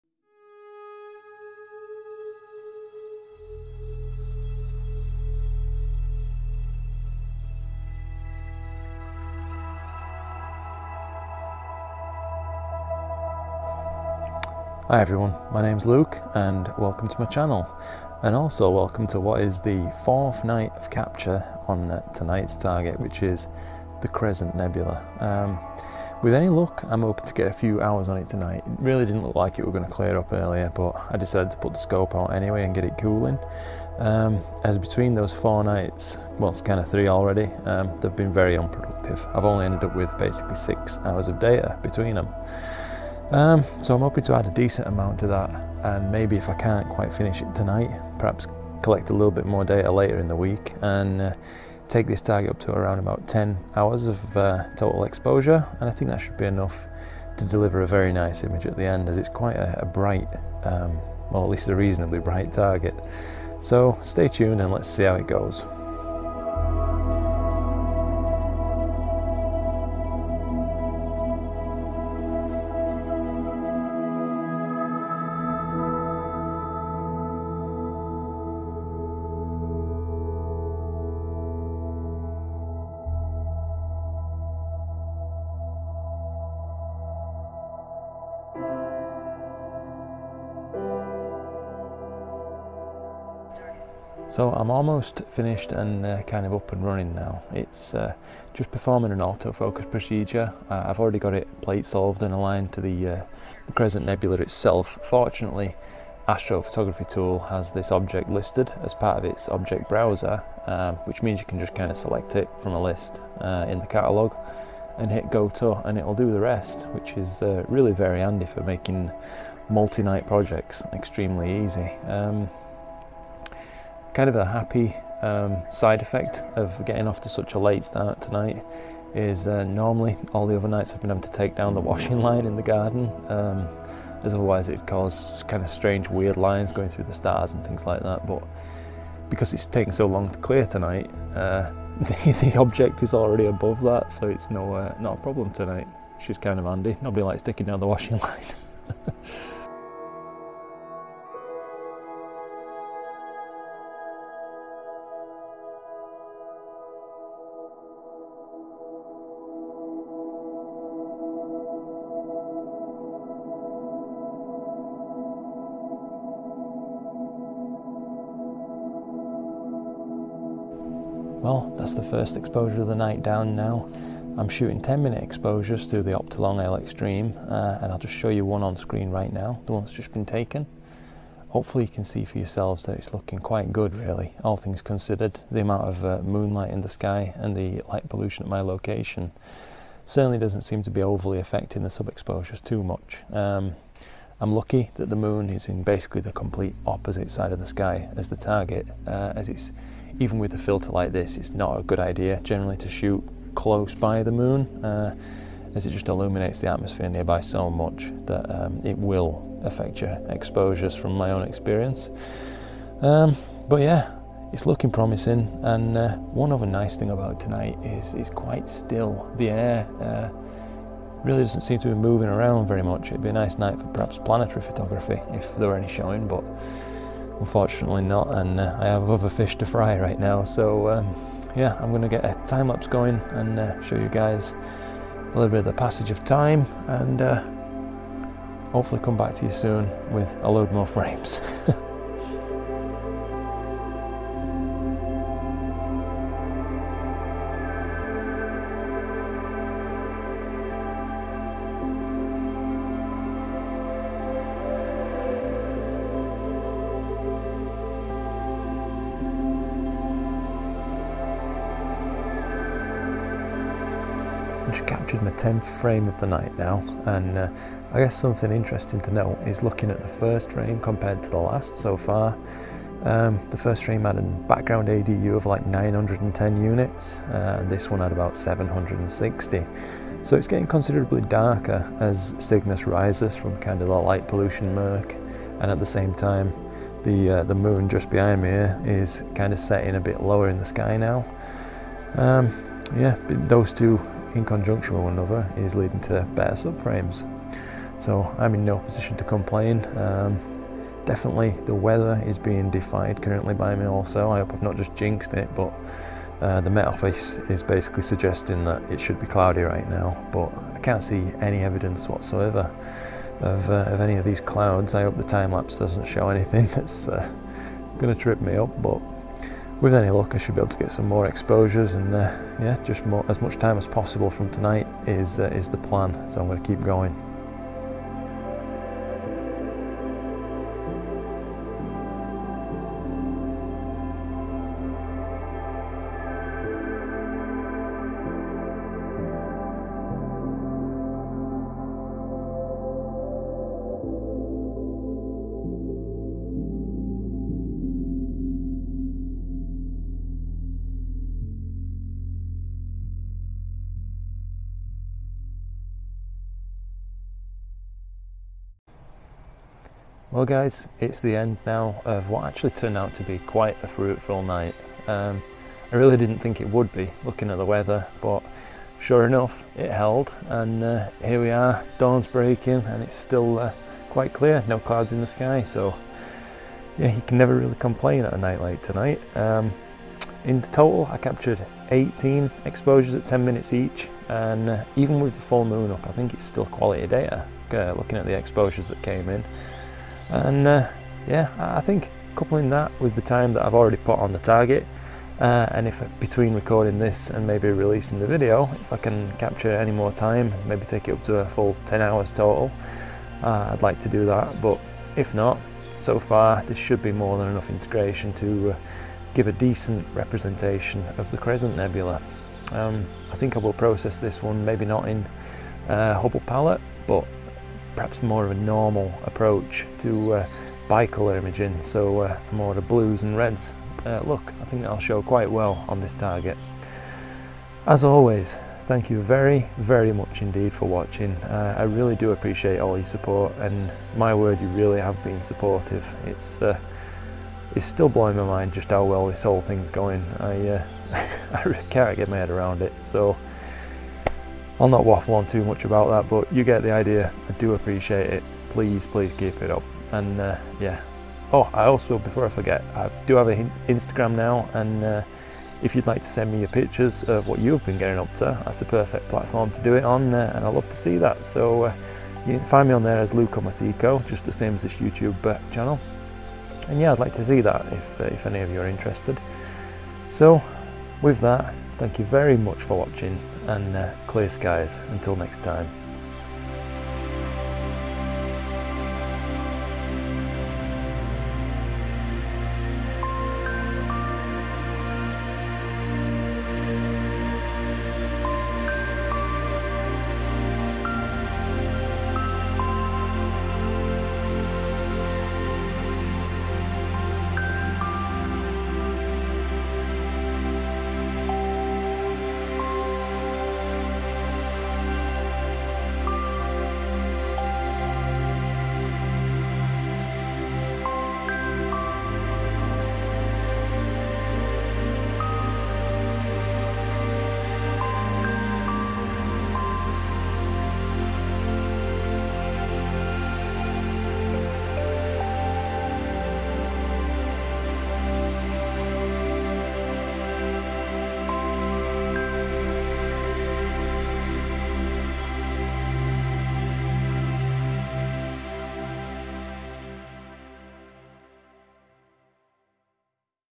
My BEST Photo of the Crescent Nebula EVER! - OSC Narrowband Astrophotography
In this video I took my best ever image of the Crescent nebula, it's long been a favorite target of mine and I like to visit it with my telescope every season, sometimes more than once!

I use my Skywatcher Esprit 120 super apo triplet scope, ASI2600mc pro colour camera, and an Optolong L-Extreme dual-narrowband filter.